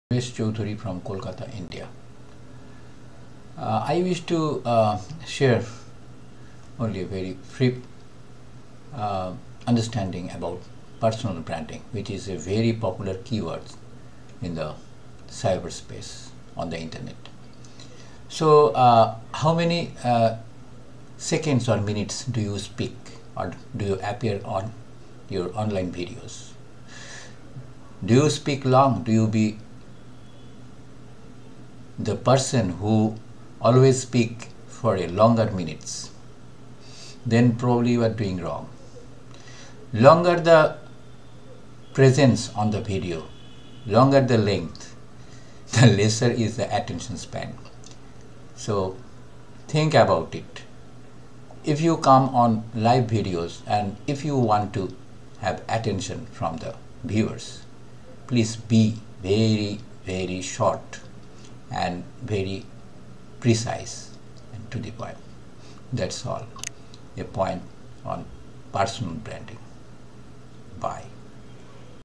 Keep Your Videos Short and Precise
How many minutes do you speak in your videos? What is the total length of your videos? Are you a person who likes to talk long and make videos longer?

You are doing something wrong.

The longer your presence in the videos, the lesser the attention span of the viewers. If you come on live videos and want attention from the viewers, be very short, precise, and to the point.

This is my two cents on building a personal brand.